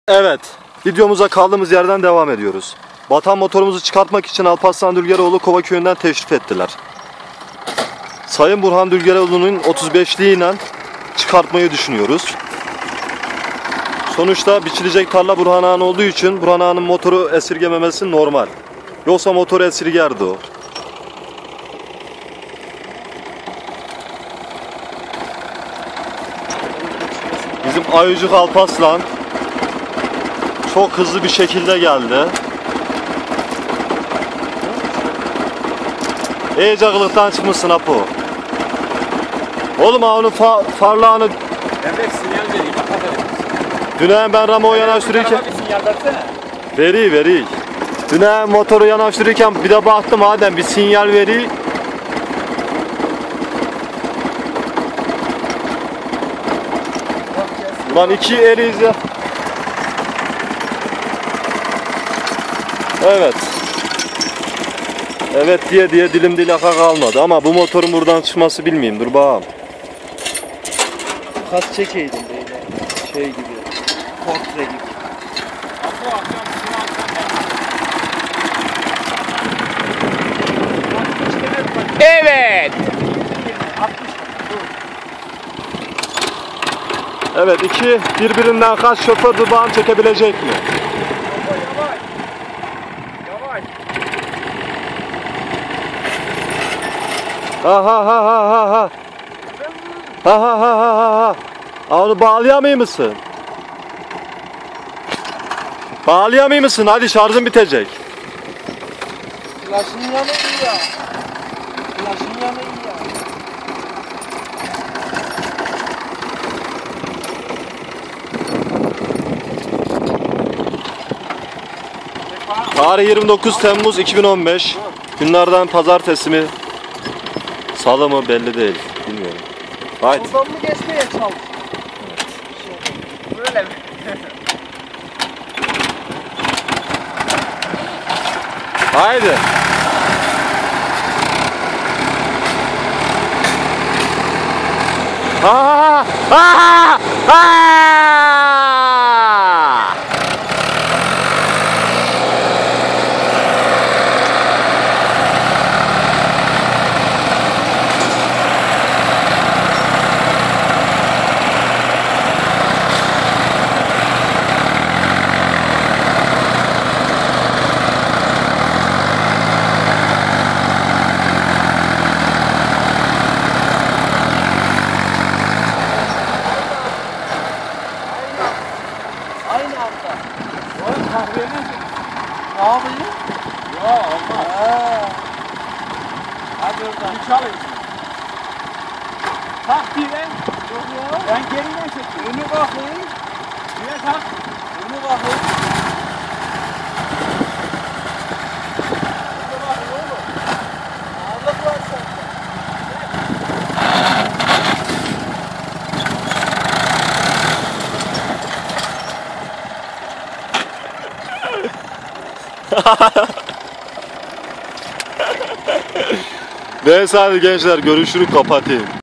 Massey Ferguson 135 lik battı başka bir 135 kurtardı
massey ferguson 135 lik batıyor başka bir massey ferguson 135lik gelip kurtardıktan sonra traktörler çekişiyor.